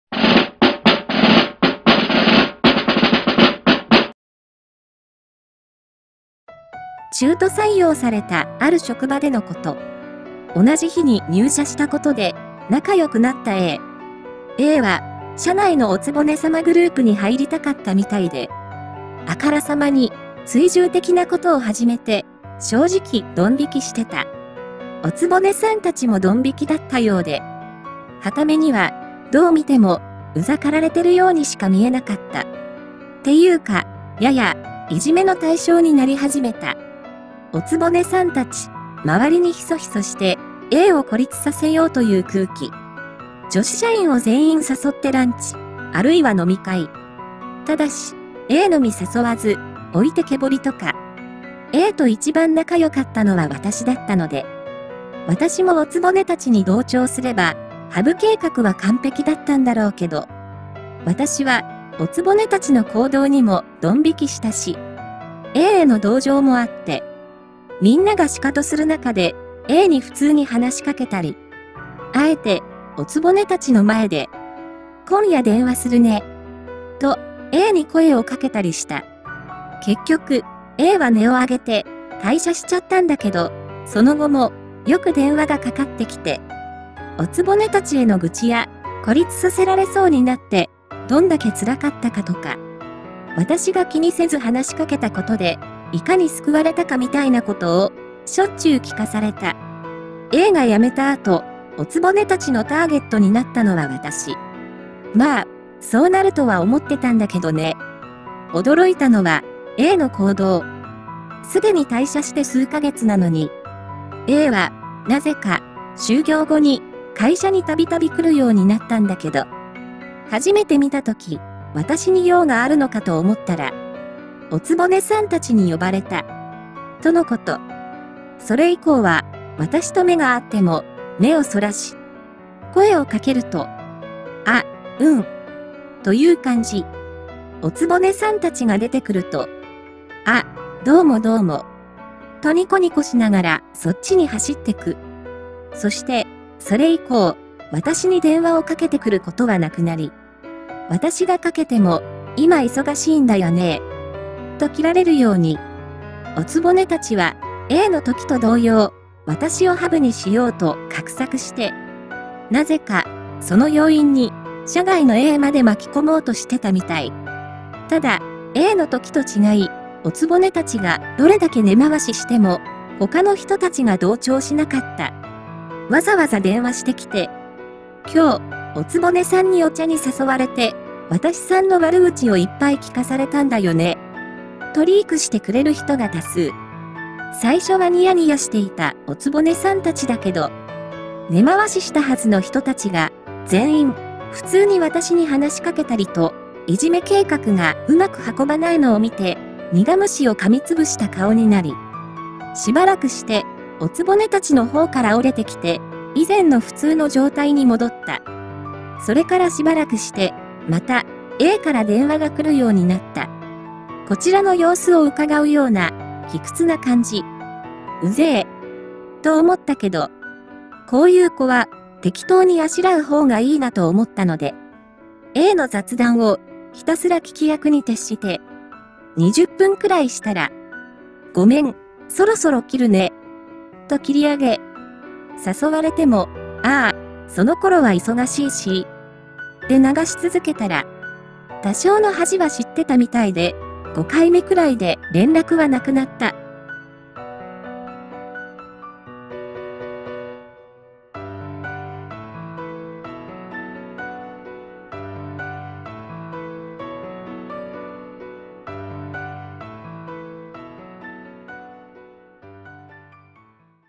お局達「こいつウザい。孤立させたろｗ」Ａ「・・会社辞める。私さんだけ声かけてくれて、嬉しかった」私「うん・・」→数カ月後、Ａを会社で見かけ・・【修羅場】
DOVA-SYNDROME